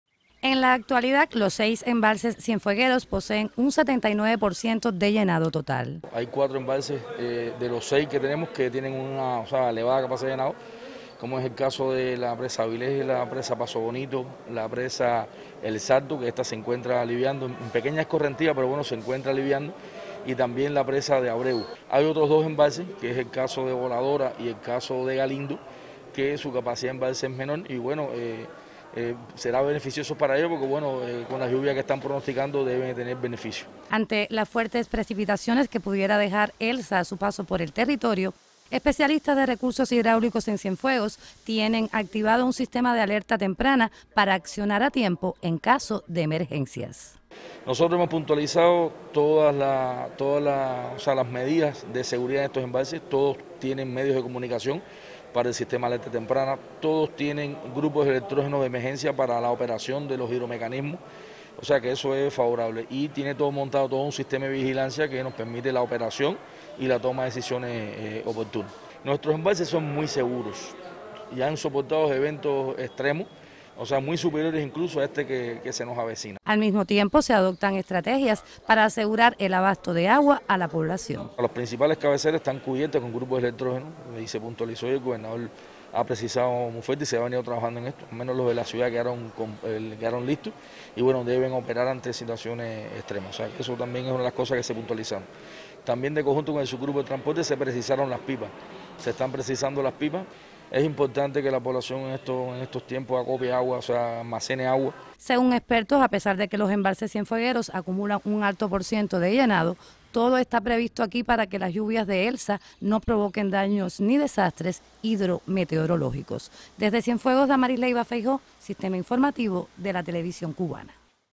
Cobertura Elsa.
Los embalses cienfuegueros poseen una elevada capacidad de llenado. Ante la proximidad del evento meteorológico Elsa  se puntualizan en el territorio acciones para activar el sistema de vigilancia  y la toma de decisiones oportunas.